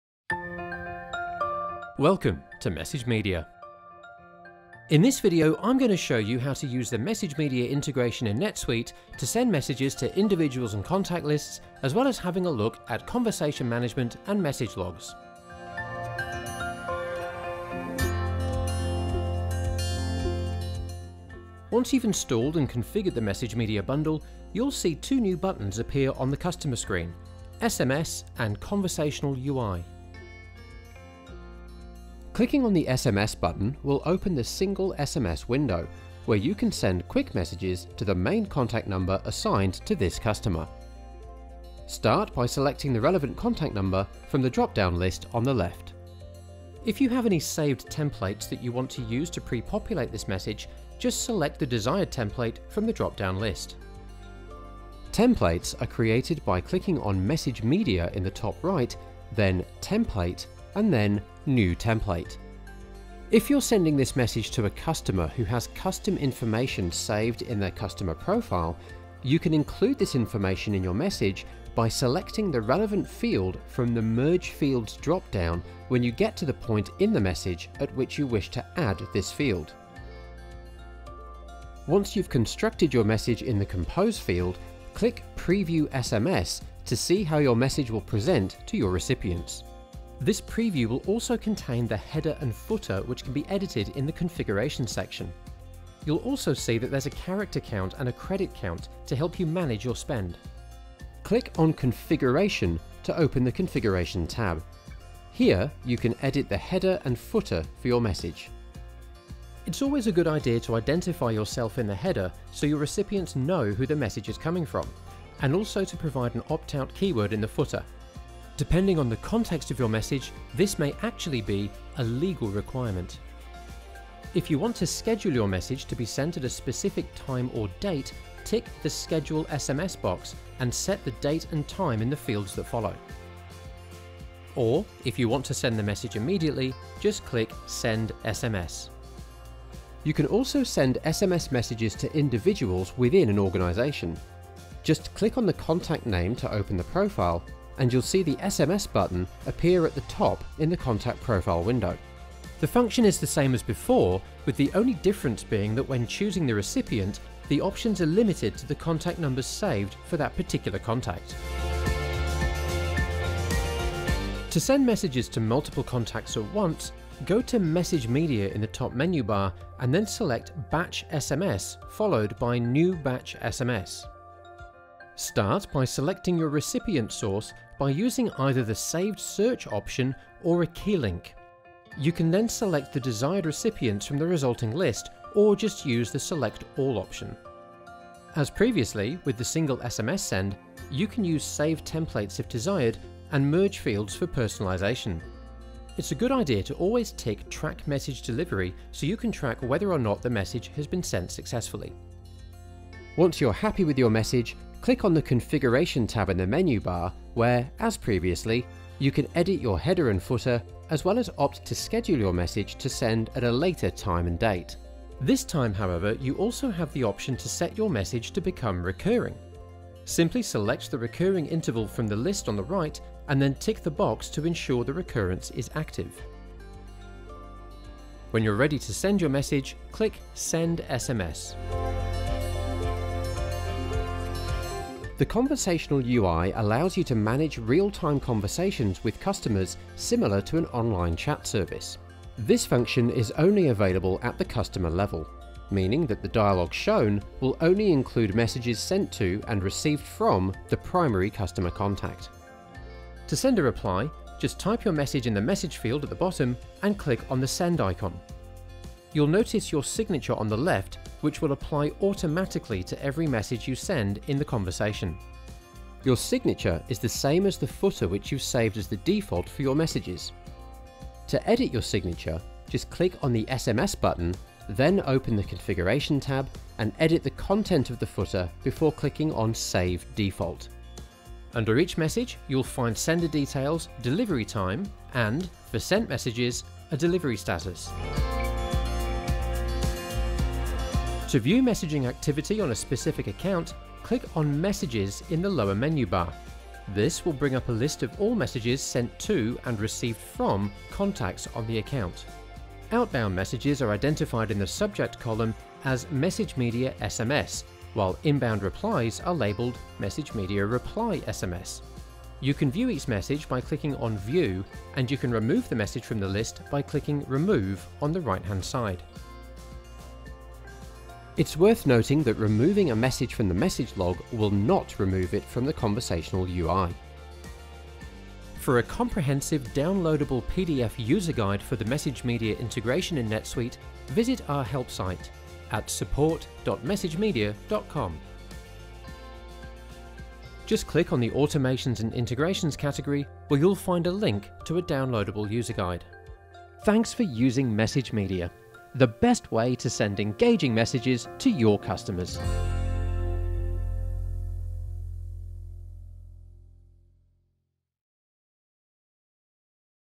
How to use the MessageMedia integration in Netsuite
A short instructional video to show you how to use the MessageMedia integration in Netsuite to send single and batch messages, manage conversations, and view message logs.